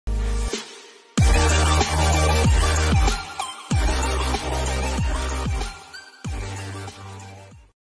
My new outro! (Read desc)
Since @VillageOfObjects is still working on voo16b and it’s coming in August I think ill try to add perfume the outro

I hope you like it @VillageOfObjects